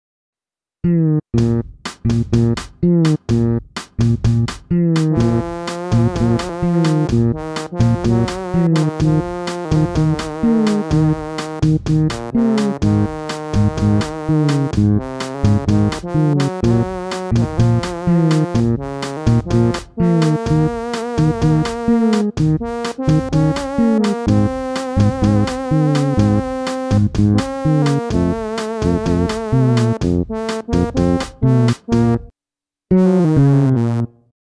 OP-1 como trombone
Tocando uma conhecida introdução de Los Hermanos com o OP-1 (cluster synth) e o iFretless Bass para iPad.